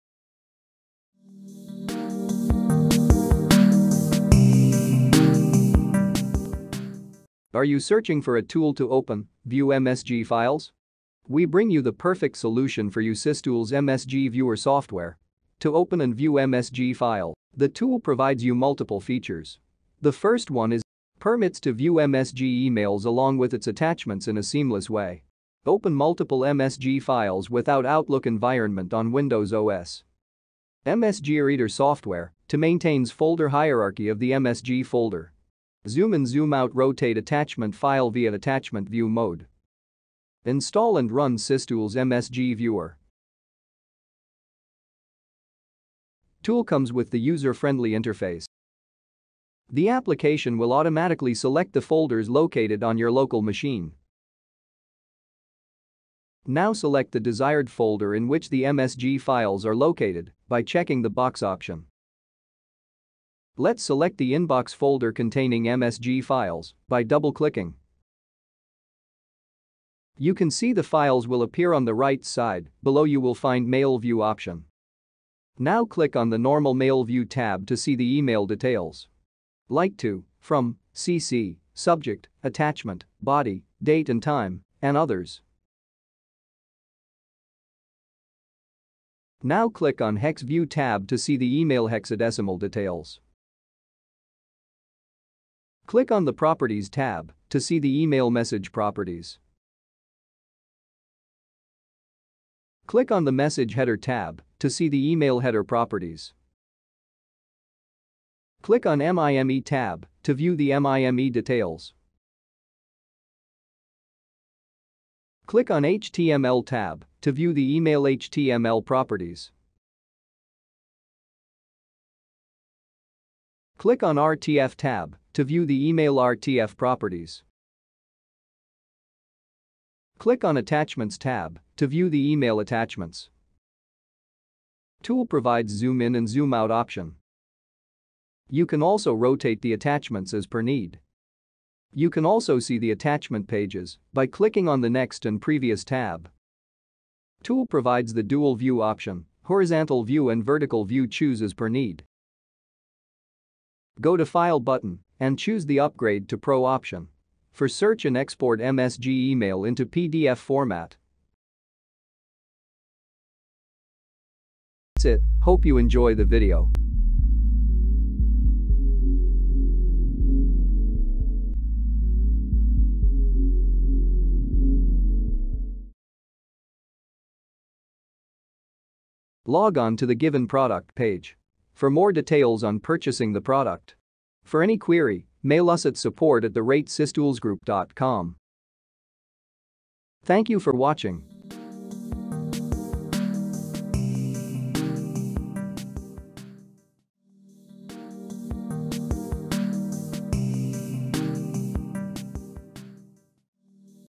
Free MSG Viewer Tool by SysTools | Open MSG Files without Outlook | Best MSG Reader Software
SysTools MSG viewer software is a standalone utility to open MSG file and show all attribute. Its easy to use UI help users to perform the operations easily.

Have a bunch of MSG files and looking for a solution to open MSG files without Outlook. To view MSG files, you can download the free MSG viewer tool for Windows OS 11, 10, 8, and 7, Windows Server 2019, 2016, 2012, and 2008 versions.

SysTools MSG Viewer Tool to open multiple MSG files without the need for Outlook 2021, 2019, 2016, 2013, 2010, 2007, 2003, 2002, and 2000 versions. Easily read MSG email messages with associated attachments. View Outlook messages in Normal, Hex, Properties, Message Header, MIME, HTML, RTF, and Attachments modes.
Steps to Open MSG File using SysTools Software
Step-1: Download and Install The SysTools MSG Viewer
Step-2: Launch The Software and Add MSG File / Folder
Step-3: Preview Entire Data into Software Panel
Timeline:
0:00 Video Introduction
0:18 Main Features
0:50 Explore Folder
1:10 View MSG File Data
2:38 Upgrade to Pro